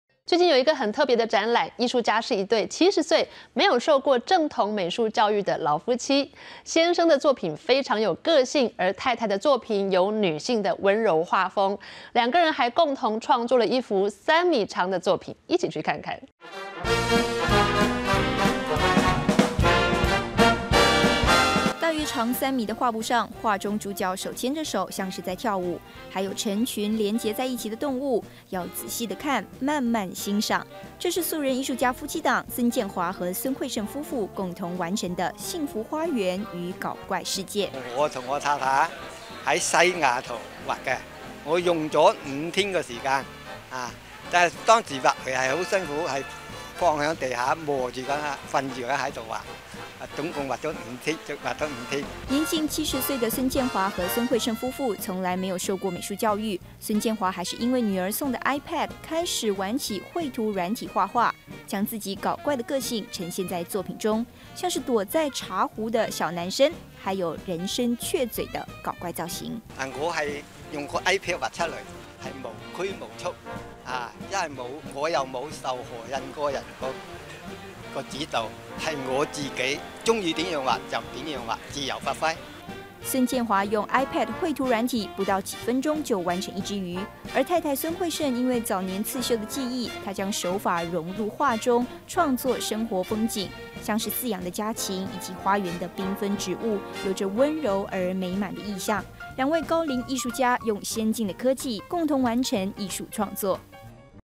《藝想世界》70歲素人藝術家 夫妻檔畫出搞怪世界
最近有個很特別的展覽，藝術家是一對年近70，沒有受過正統美術教育的老夫妻。先生的作品非常有個性，有時候人物躲在茶壺中，有時候幻化成像是麻雀的嘴，他創作了屬於自己的搞怪世界。而太太的畫風如女性般溫柔，兩人還共同創作一副3米長的作品。Facebook粉絲團